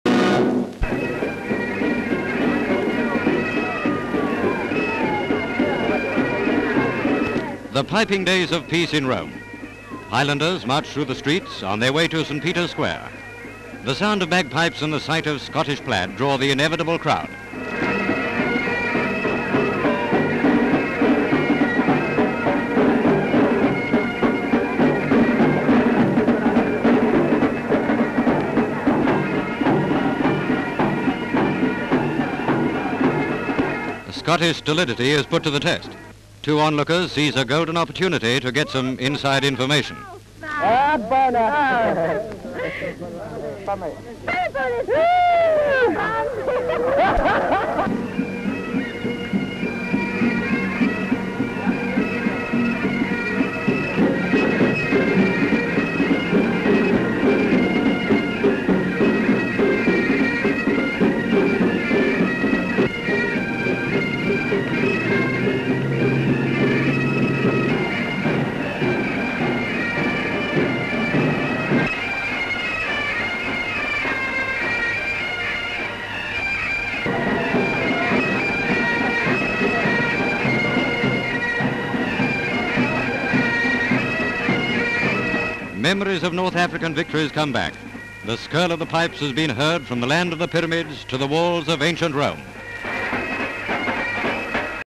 In Rome Today (1944)
Title reads: "In Rome Today".

Italy.

Various shots of band of pipers of a Highland Regiment marching through St. Peter's Square with crowds watching them. M/S of two saucy young Italian girls trying to look up the kilts of two bashful Scottish soldiers.  Lots of squealing and giggling! Various shots of the marching off past the Colosseum and through more streets.
 FILM ID:1360.06